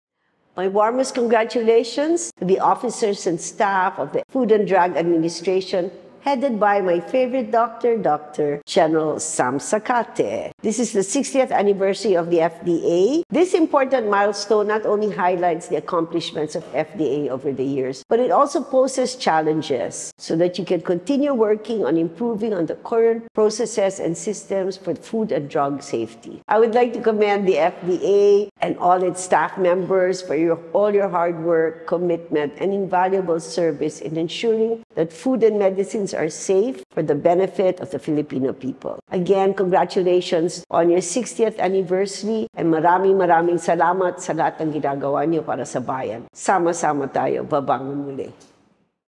First Lady Liza Marcos greets FDA Philippines on its 60th Founding Anniversary
Pagbati mula kay First Lady Marie Louise "Liza" Araneta Marcos para sa ika-60 anibersaryo ng Food and Drug Administration (FDA).